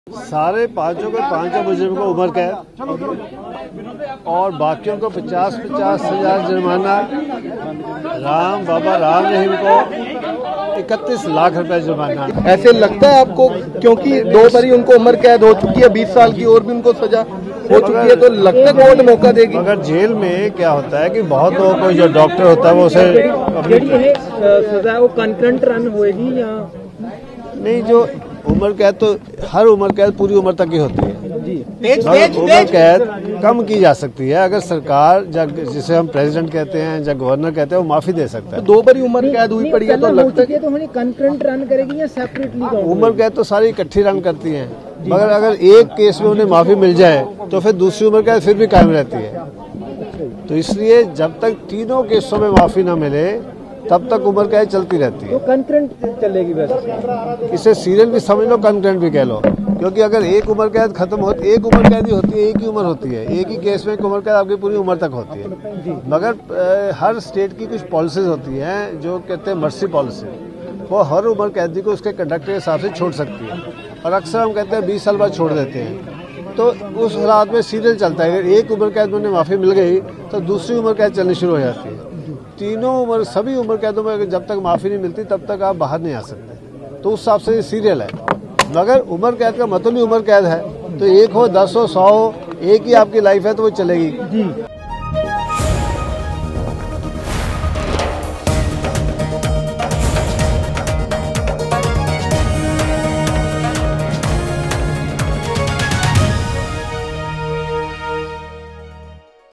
Ram Rahim Case: राम रहीम को रणजीत सिंह हत्याकांड में उम्रकैद
रणजीत सिंह हत्याकांड में  सीबीआई की विशेष अदालत (CBI Special Court) डेरा सच्चा सौदा के प्रमुख गुरमीत राम रहीम (Ram Rahim) समेत 5 दोषियों को उम्रकैद की सजा सुनाई है. राम रहीम को इससे पहले पत्रकार रामचंद्र छत्रपति हत्याकांड में भी उम्रकैद की सजा हो चुकी है.